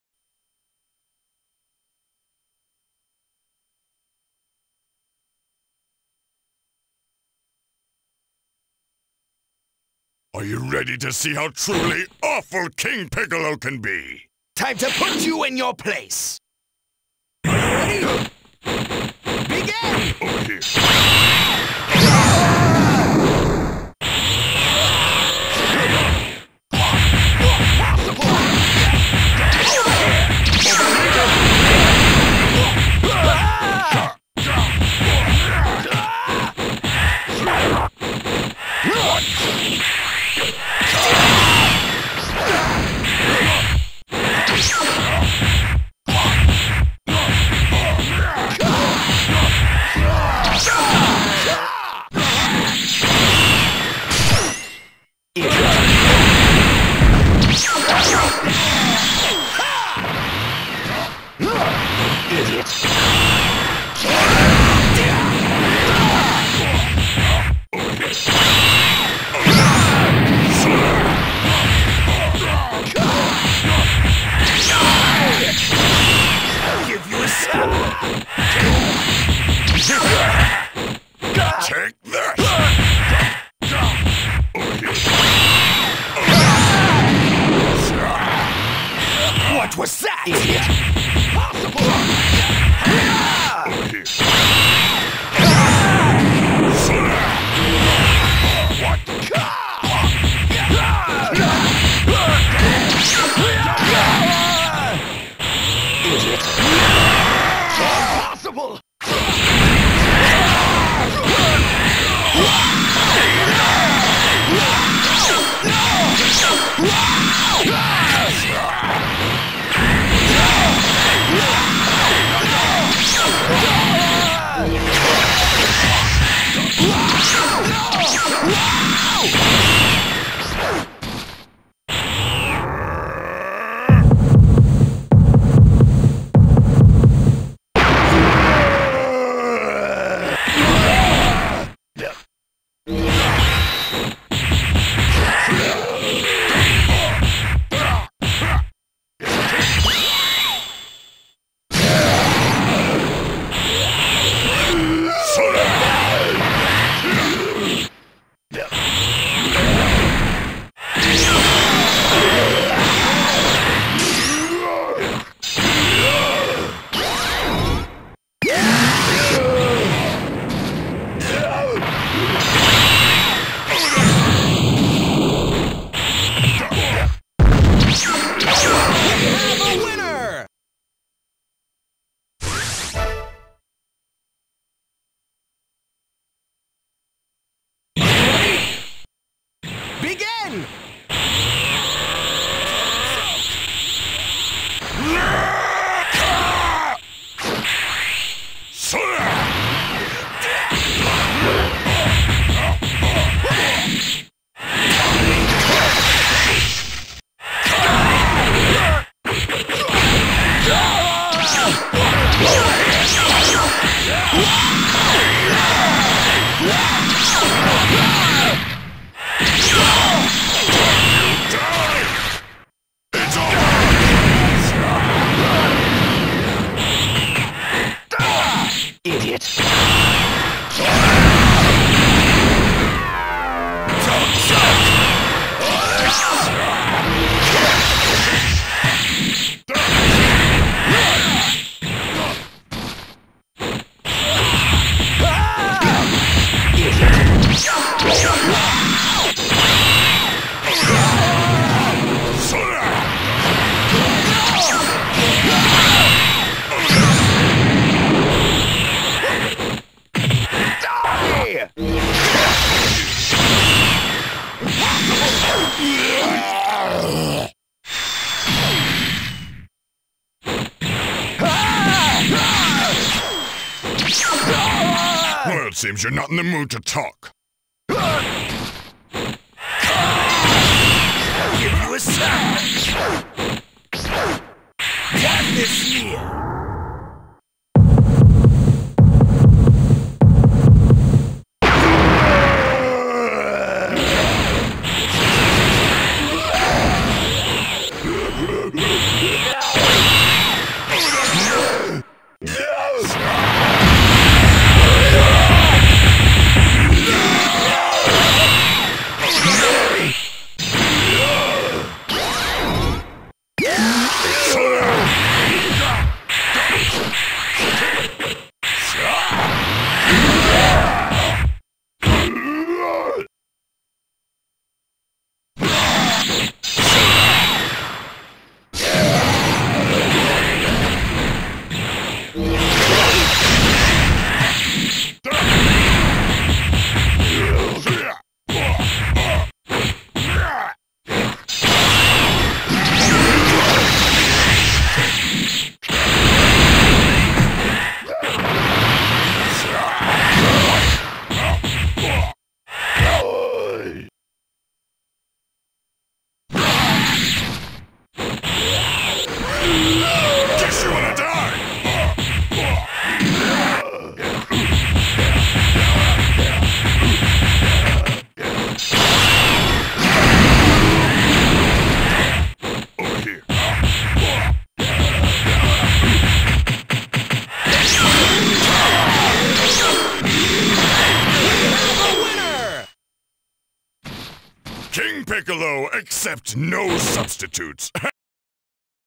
King Piccolo vs Raditz [Alt]
[King Piccolo]
-Fighting Spirit: Ki power rises as health decreases
-Emperors Aura: Max power mode lasts longer, increase in power to stun opponent
-A.I. Type: Cell

[Raditz]
-Serious: Atk rises as health decreases
-Combo Master: Combos and barrage attacks cause more damage
-Unleash Ki: Small additional advantage in clashes
-A.I. Type: Majin Buu